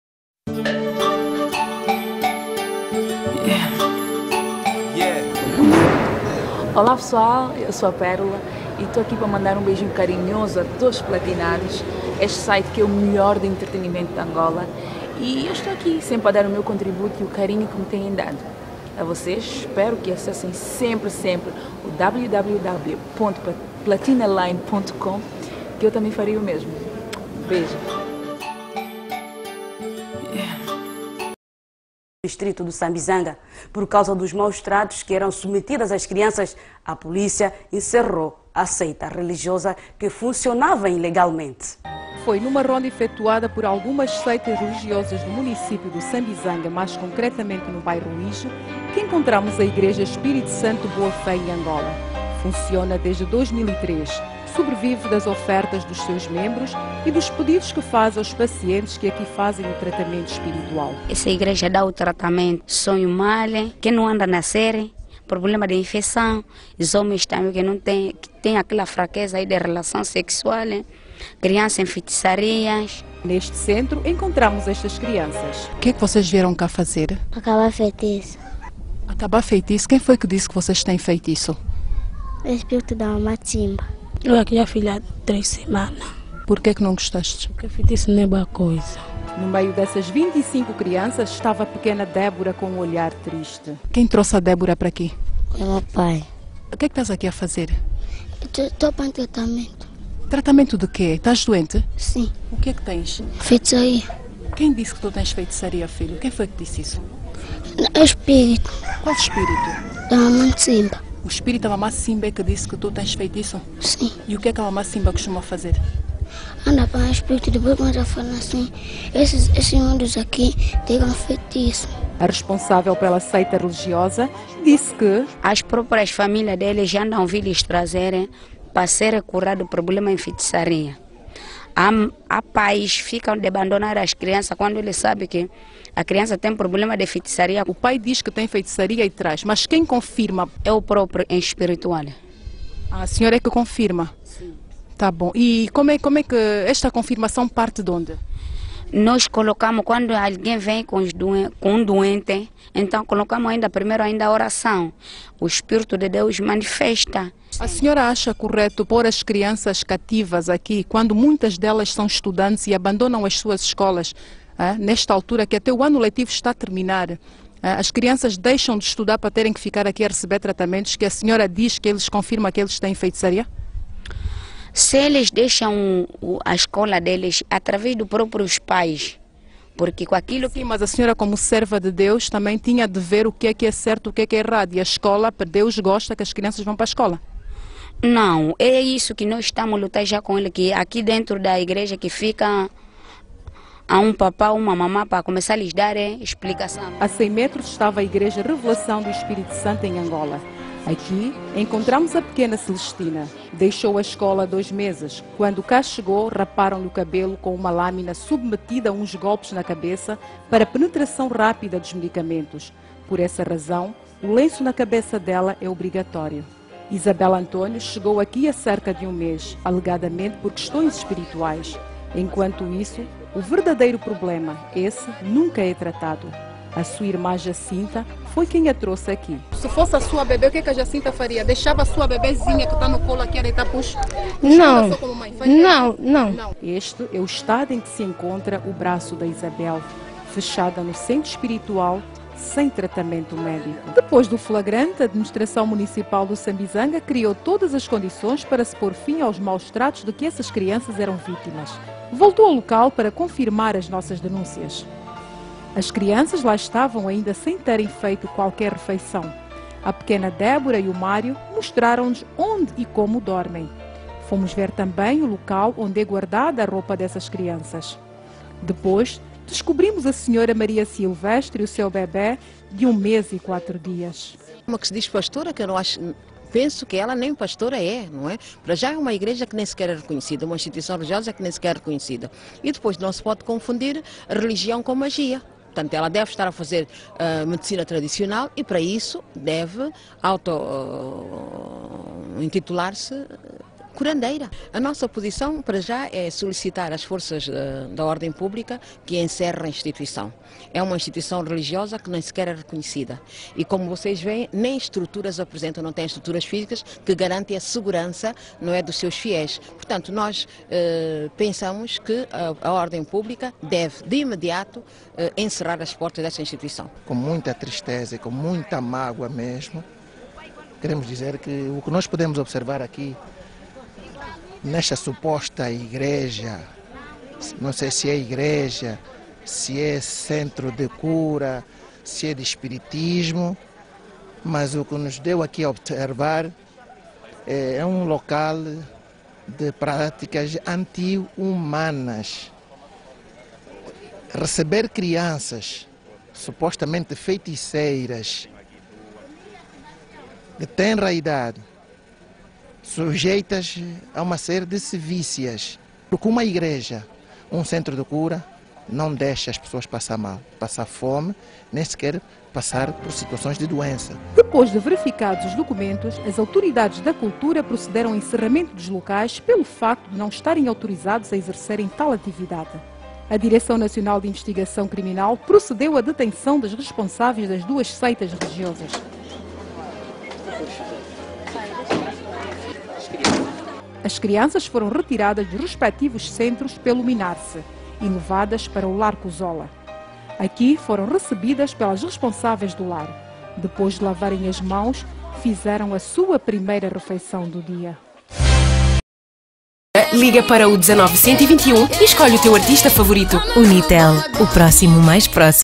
Reportagem Mostra Igreja que promete soluções Milagrosas
a reportagem mostra a igreja que foi encerrada pois prometia,  virilidade aos homens, expulsão de feitiço, soluções de gravidez. e muito mais veja reportagem